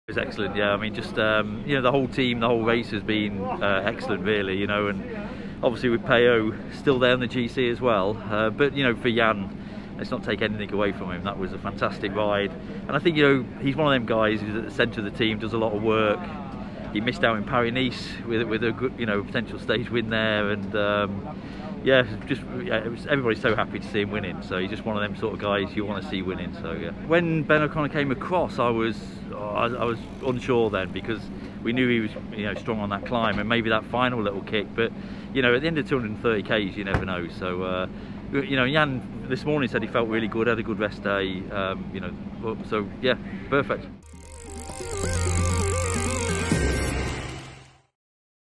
Giro d'Italia 2020: Rod Ellingworth on Jan Tratnik's win
Bahrain-McLaren's Rod Ellingworth reacts to Jan Tratnik's Stage 16 win at the Giro d'Italia.